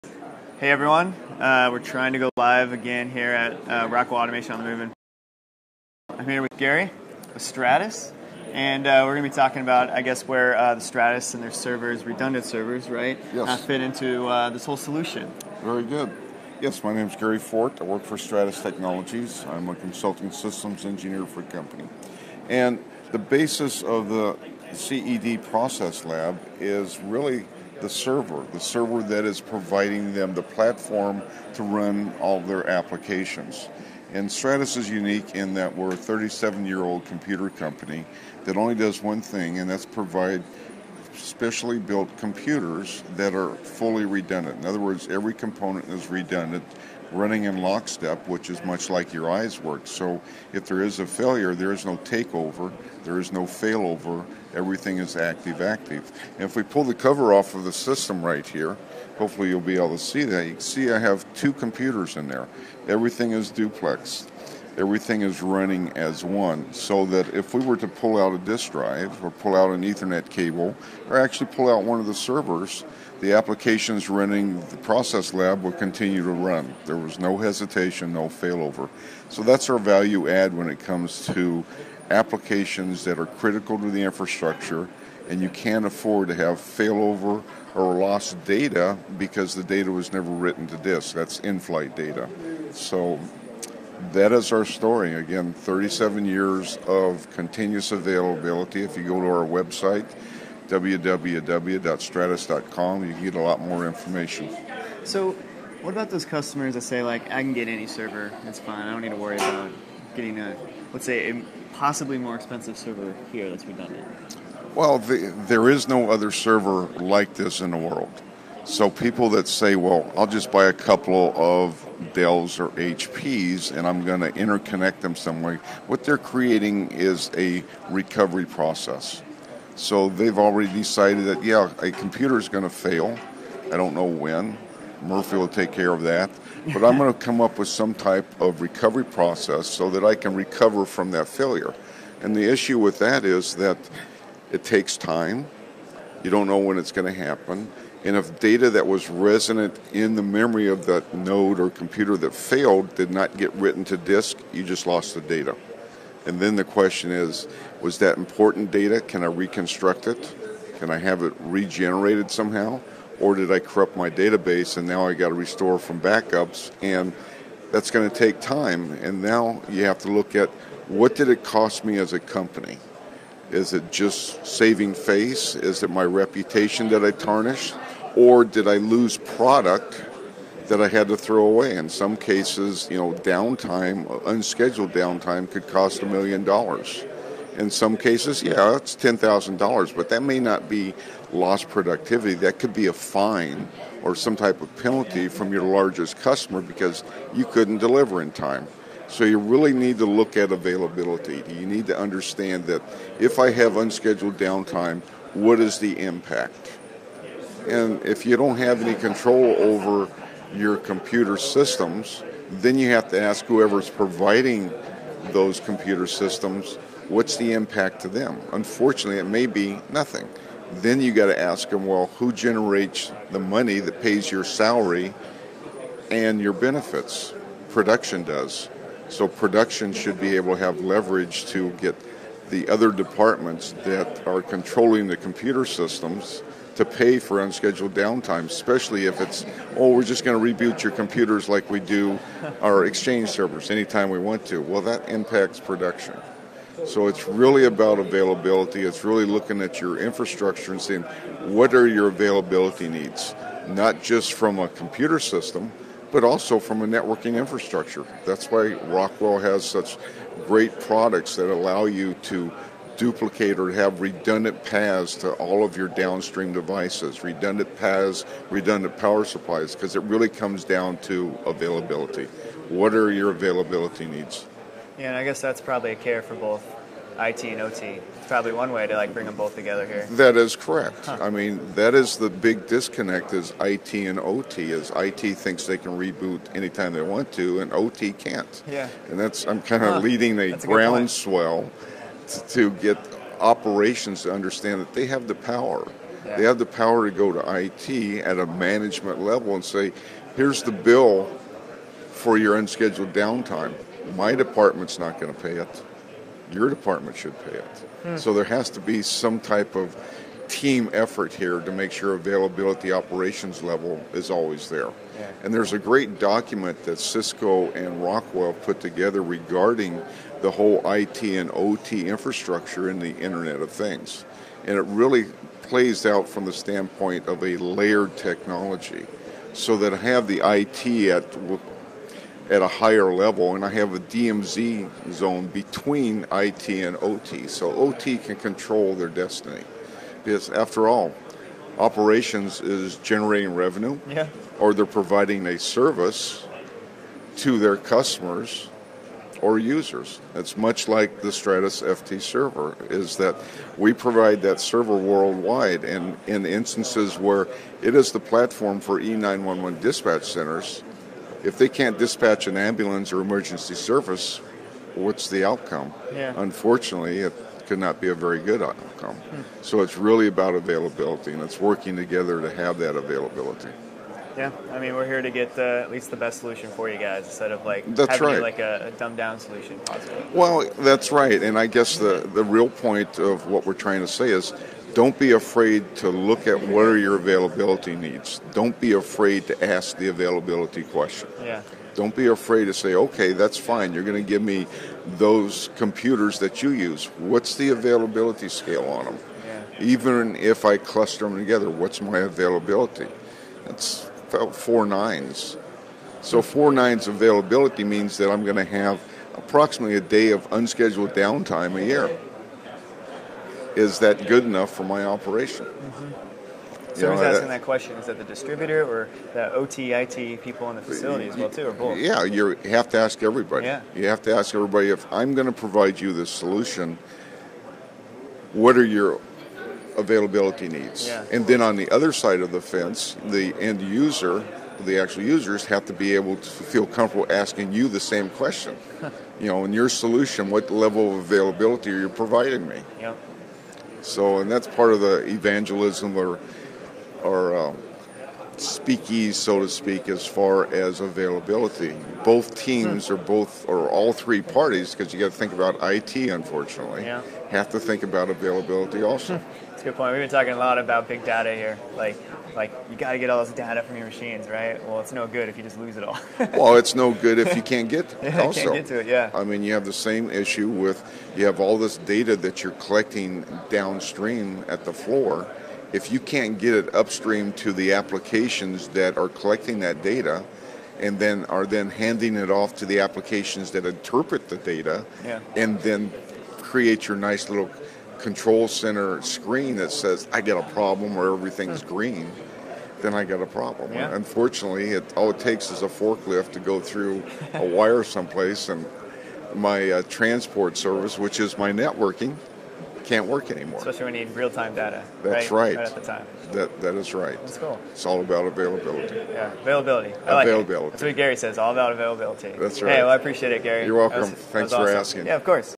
Stratus Fully Redundant Servers are Critical to Your Infrastructure
Learn from Stratus on why your server and having a redundant server is critical to securing your data and mitigating any downtime.